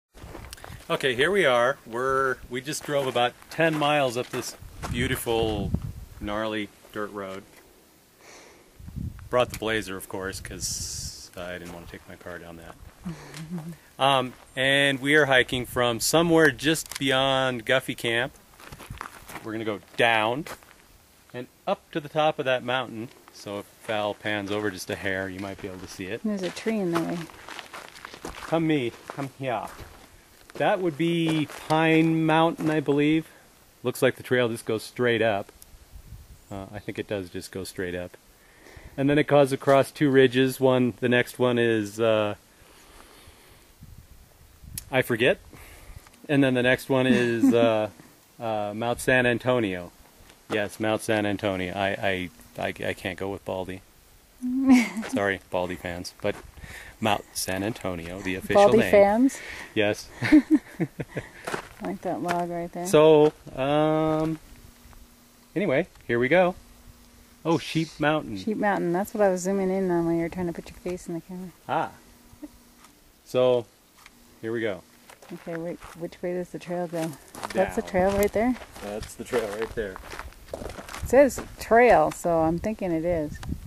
Hike August 13th, 2011
Hiking up to Pine Mountain